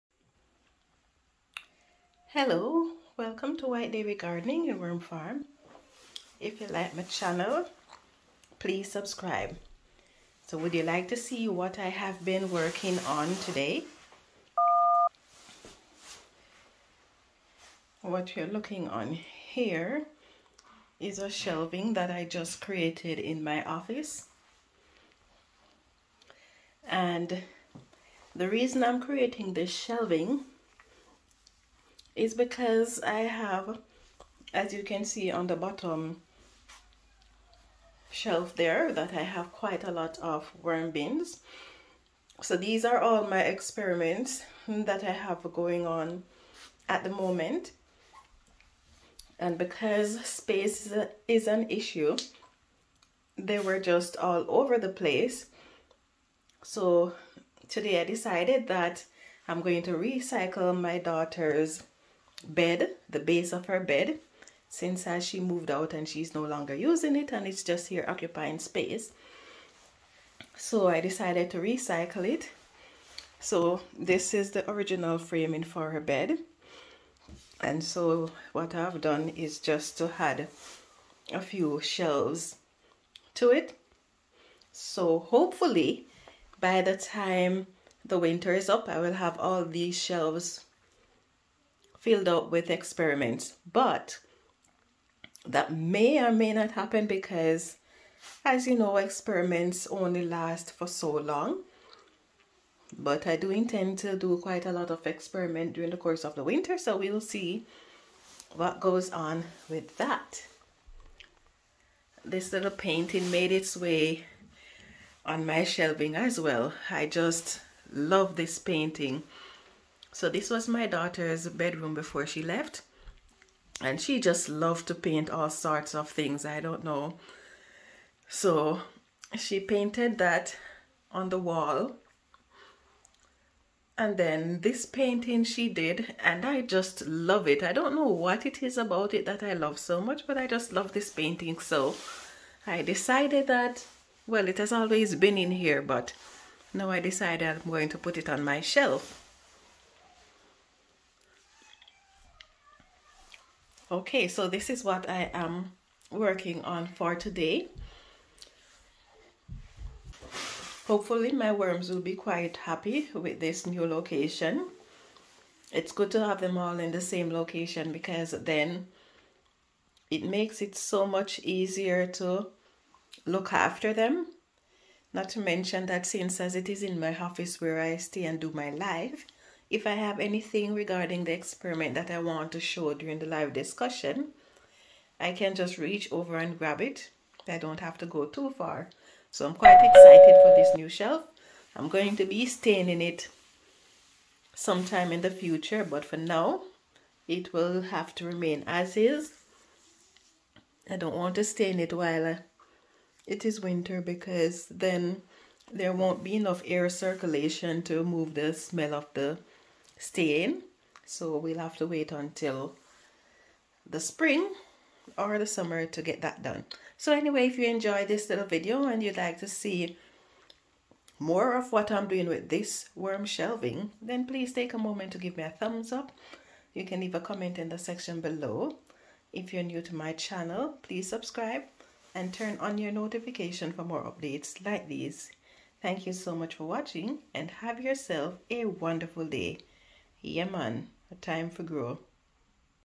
Experimental Worm Bins Get Their Own Home
My experimental worm bins are kept indoors for the winter and they need a home. Check out what I did for them.